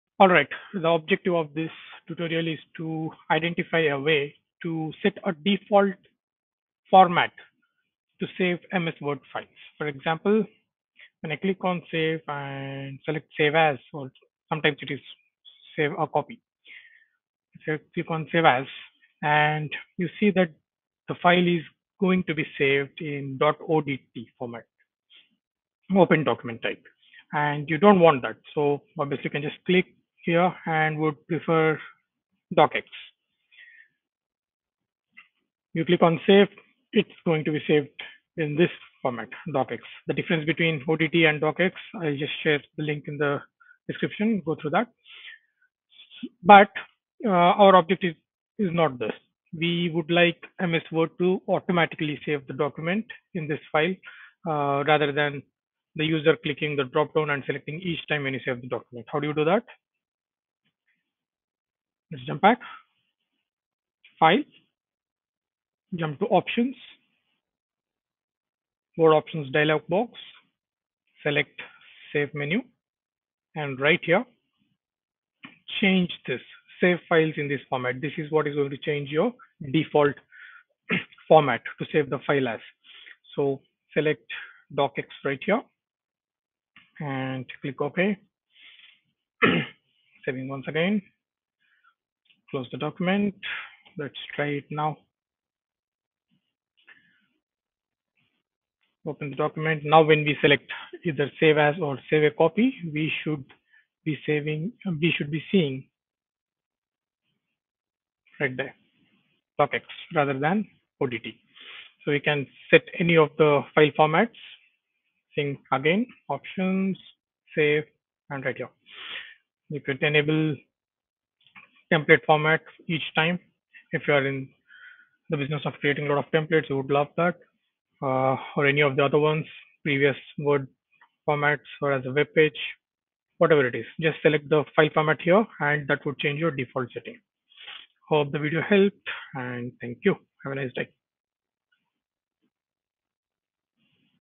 Set default saving file format in MS Word
Discover how to streamline your Microsoft Word experience by setting your preferred default file format effortlessly. Follow our step-by-step instructions to customize your settings, save valuable time, and simplify your document-saving process.